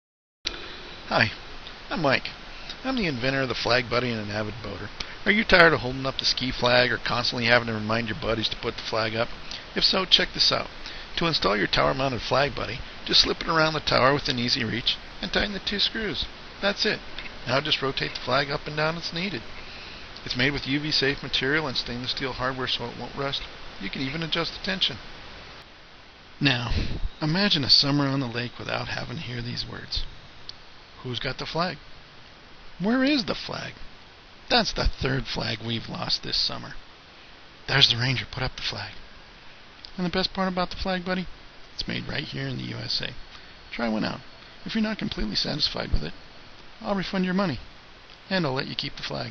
Wakeboard tower Mounted Flag Holder.wmv
Tired of holding the ski flag? Just rotate the flag up and down as needed!
Flagbuddy.com .I have a window mount as well.